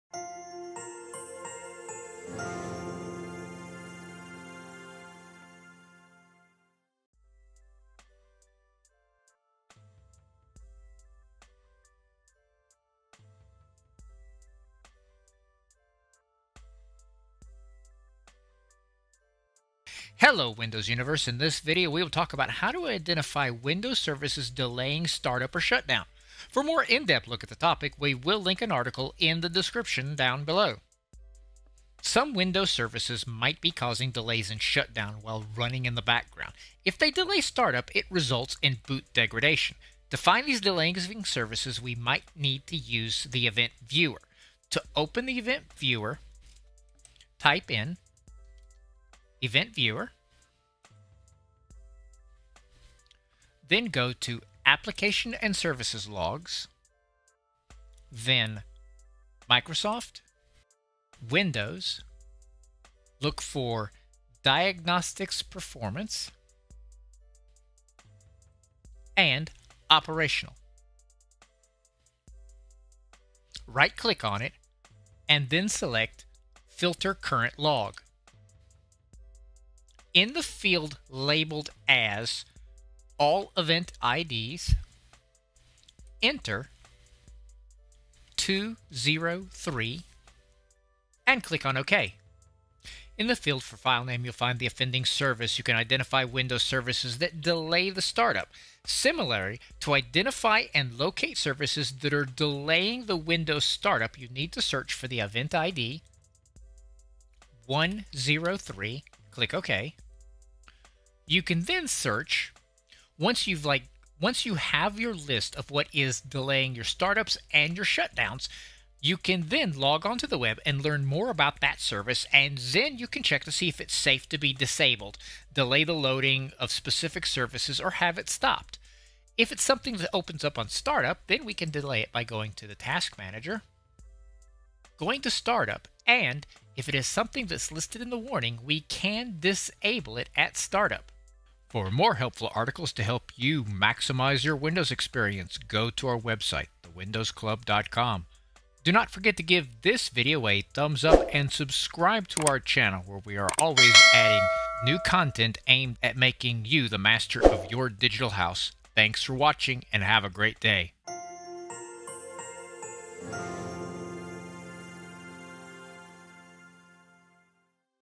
How to identify Windows Services delaying Shutdown or Startup
We all have read articles online about speeding up our Windows computers. In this tutorial, I am focusing on one item that can cause your Windows computer to shut down or start up slowly – and those are Windows Services. If some Service refuses to terminate or takes a long time to terminate, then the system shutdown gets delayed. Many times you just wait in front of your computer for it to either startup or shut down. This is irritating at times. But at the same time, during a shutdown, it is necessary for your computer to properly close all the programs and processes just to make your next startup better.